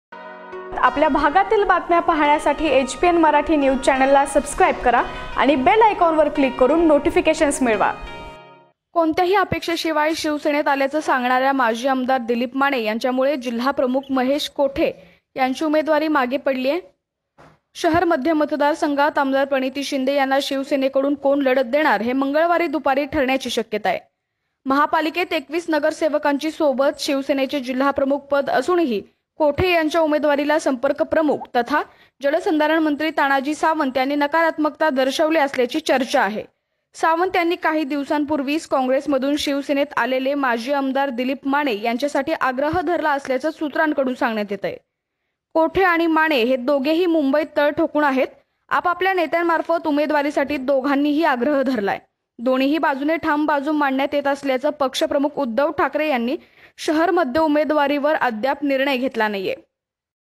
सोलापूर मध्यमधून शिवसेनेकडून महेश कोठे की दिलीप माने?
कोणत्याही अपेक्षेशिवाय शिवसेनेत आल्याचे सांगणाऱ्या माजी आमदार दिलीप माने यांच्यामुळे जिल्हाप्रमुख महेश कोठे यांची उमेदवारी मागे पडली आहे. शहर मध्य मतदार संघात आमदार प्रणिती शिंदे यांना शिवसेनेकडून कोण लढत देणार हे मंगळवारी दुपारी ठरण्याची शक्‍यता आहे.

महापालिकेत 21 नगरसेवकांची सोबत, शिवसेनेचे जिल्हाप्रमुख पद असूनही कोठे यांच्या उमेदवारीला संपर्कप्रमुख तथा जलसंधारणमंत्री तानाजी सावंत यांनी नकारात्मकता दर्शविली असल्याची चर्चा आहे. सावंत यांनी काही दिवसांपूर्वी कॉंग्रेसमधून शिवसेनेत आलेल्या माजी आमदार दिलीप माने यांच्यासाठी आग्रह धरला असल्याचे सूत्रांकडून सांगण्यात येत आहे.

 कोठे आणि माने हे दोघेही मुंबईत तळ ठोकून आहेत. आपापल्याला नेत्यांमार्फत उमेदवारीसाठी दोघांनी आग्रह धरला आहे. दोन्ही बाजूने ठाम बाजू मांडण्यात येत असल्याचे पक्षप्रमुख उद्धव ठाकरे यांनी शहर मध्यच्या उमेदवारीवर अद्याप निर्णय घेतला नाही.